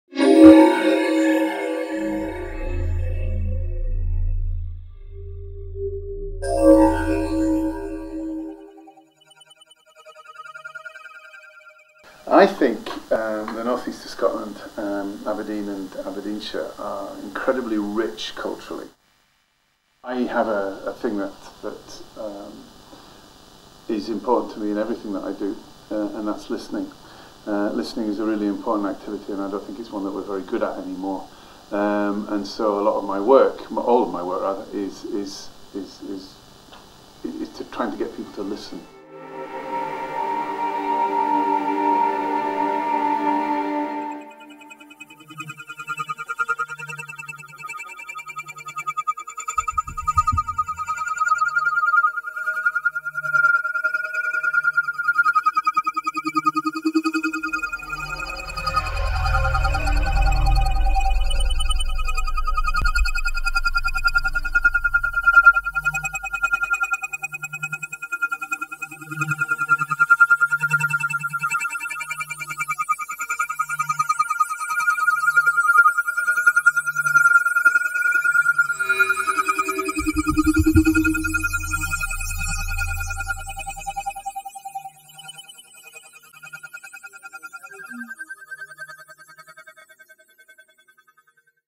Pete Stollery
90 second film on the Aberdeen based composer, teacher and performer Pete Stollery. Pete works almost exclusively in the 'electroacoustic' medium. This film is part of the 'Made In Aberdeen' 2014 film series, showcasing the wealth of creative talent working in Aberdeen city.

A film by Sarah Pascal